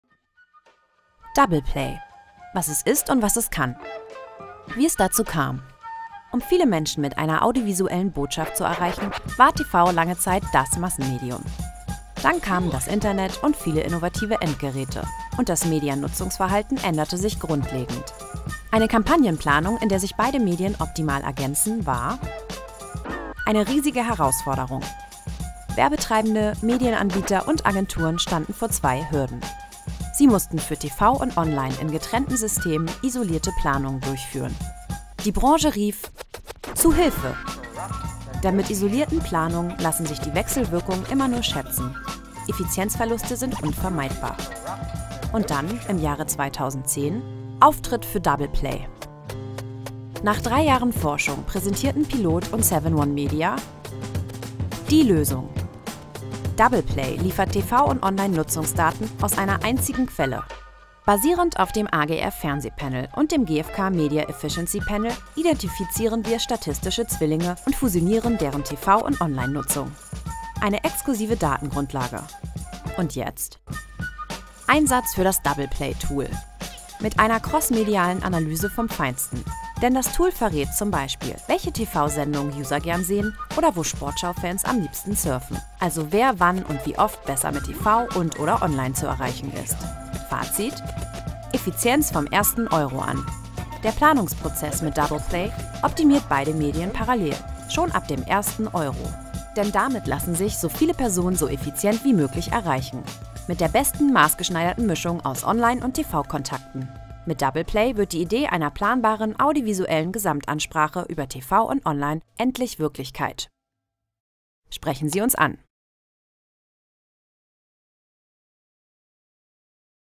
Double Play auf der dmexco 2011
Das strategische Planungsinstrument Double Play liefert TV- und Online-Nutzungsdaten aus einer Quelle. Auf der dmexco 2011 stellten pilot und SevenOne Media ihre ersten Erkenntnisse und Anwendungsbeispiele von Double Play vor.